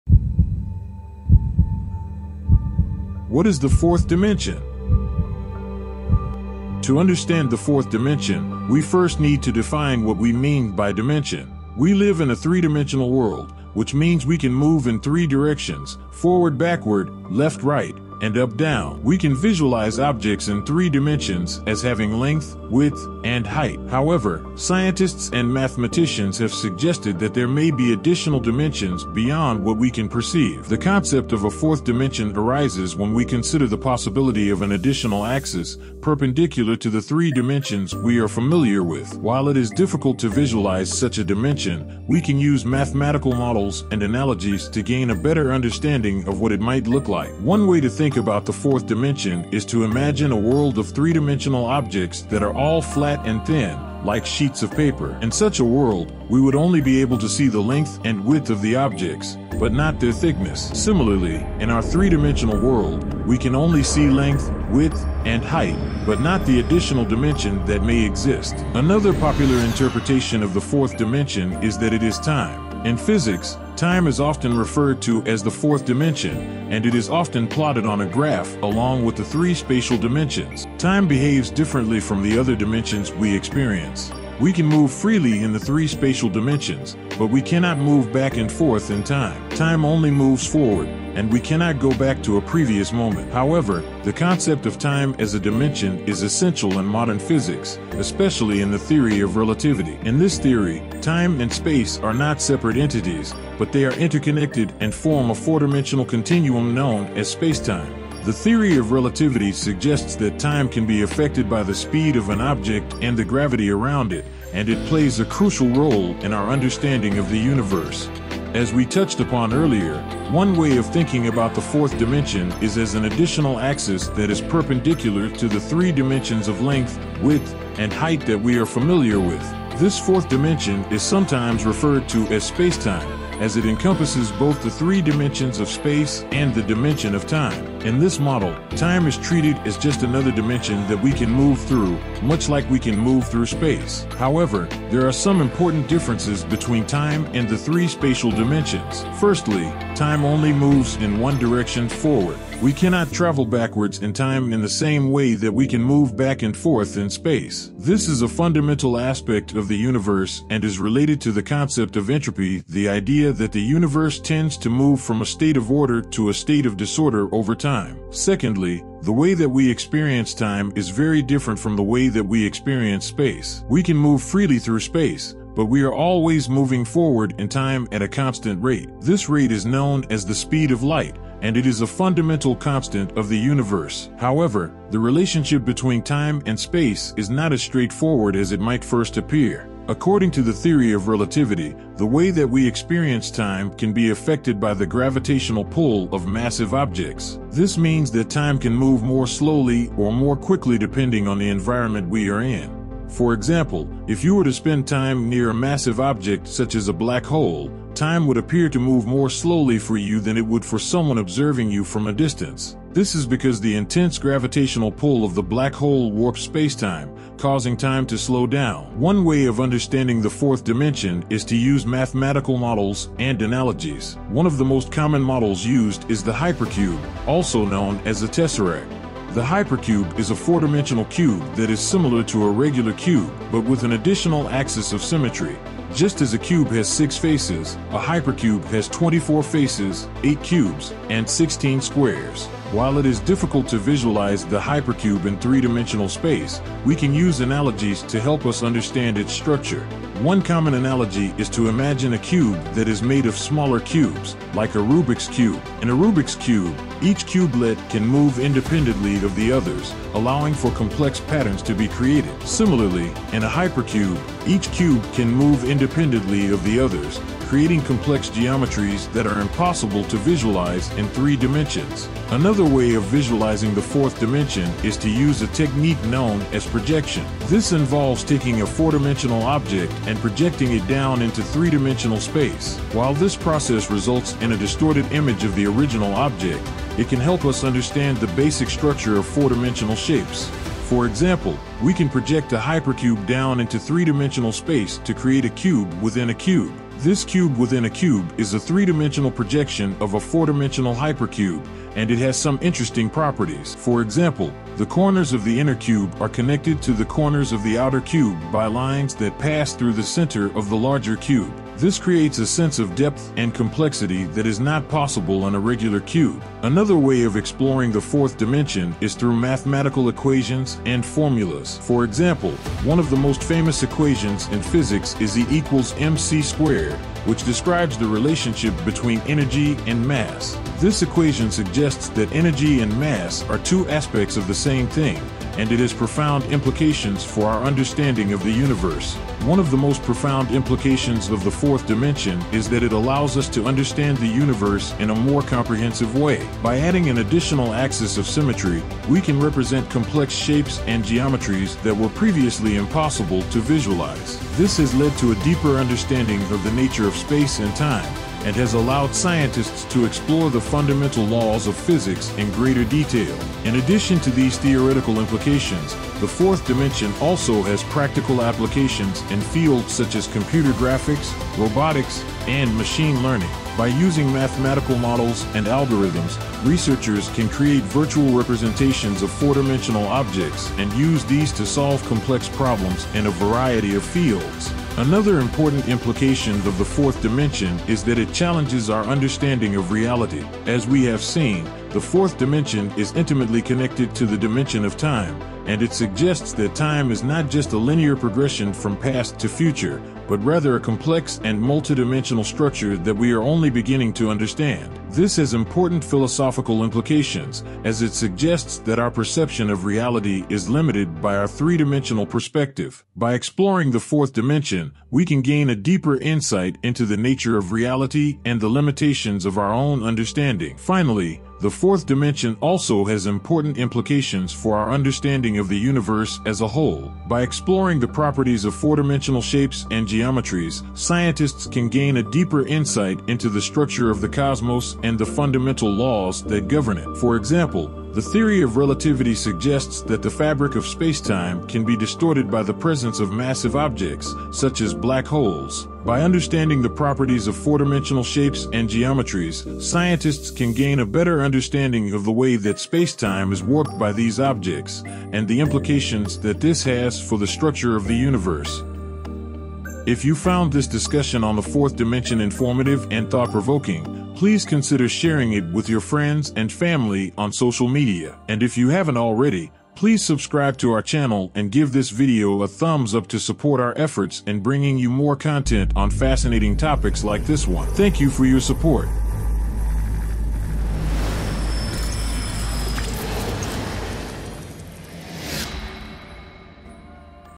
A World Beyond Our Imagination | The Fourth Dimension
While we can easily visualize the first three dimensions, the fourth dimension is a concept that challenges our understanding of space and time. From mathematical models to philosophical implications, we'll delve deep into the mysteries of the fourth dimension and its potential implications for our understanding of the universe. Get ready to expand your mind and explore a world beyond our imagination!